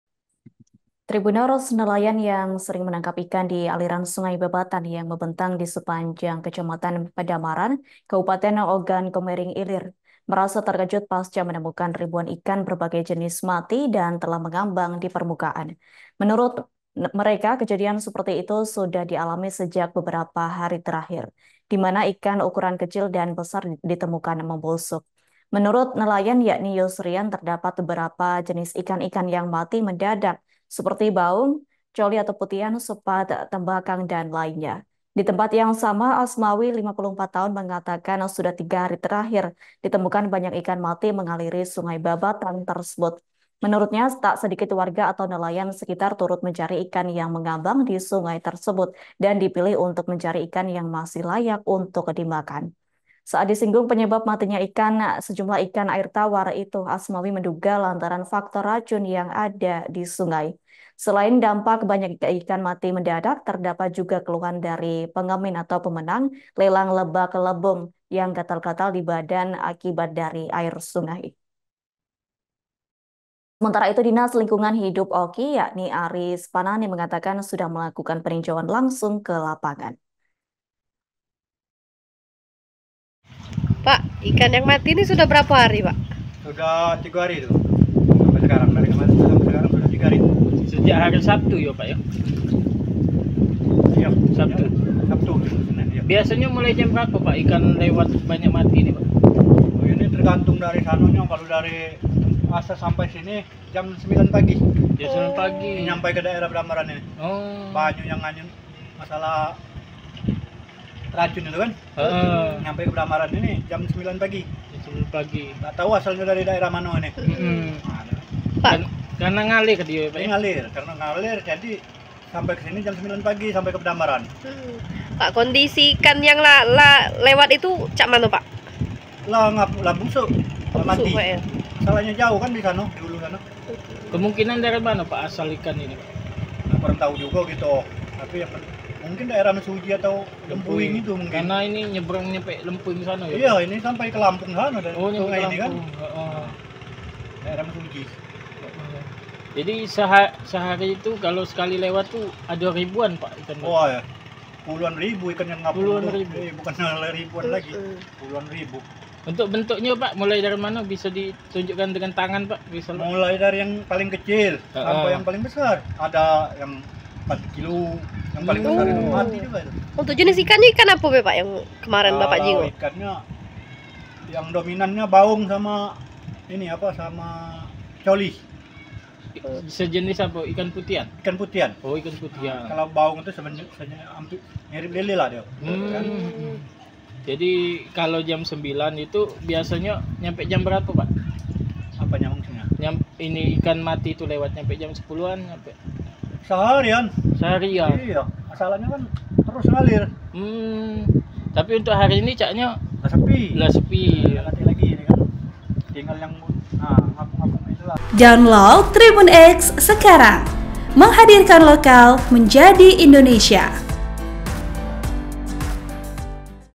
Nelayan Keluhkan Ribuan Ikan di Sungai Babatan OKI Mati Mendadak, Asmawi Menduga Faktor Racun
Laporan wartawan Tribun Sumsel - Nando
TRIBUN-VIDEO.COM- Nelayan yang sering menangkap ikan di aliran sungai babatan yang membentang di sepanjang Kecamatan Pedamaran, Kabupaten Ogan Komering Ilir merasa terkejut pasca menemukan ribuan ikan berbagai jenis mati dan telah mengambang dipermukaan.

Menurut mereka kejadian seperti ini sudah dialami sejak beberapa hari terakhir.

Di mana ikan ukuran kecil dan besar ditemukan membusuk. (*)

Program: Live Update
Host: Adila Ulfa Muna Risna
Editor Video: Latif Ghufron Aula